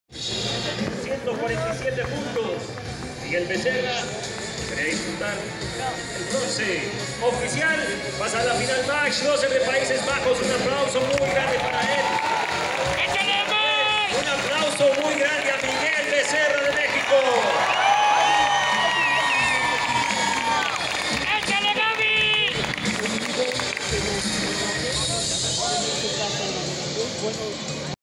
🤔| 🎯El Arquero Mexicano🇲🇽 Miguel Becerra pierde semifinal contra Mike Shcloesser de PaísesBajos🇳🇱
–El Arquero Mexicano🇲🇽 Miguel Becerra pierde en la semifinal contra Mike Shcloesser de PaísesBajos🇳🇱 aunque competirá para hacerce con la de Bronce🥉.

🌐 𝐈𝐧𝐟𝐨𝐫𝐭𝐥𝐚𝐱.𝐜𝐨𝐦
🤳🏻: 𝑨𝒏𝒆𝒕𝒕𝒆 𝑫𝒊𝒂𝒛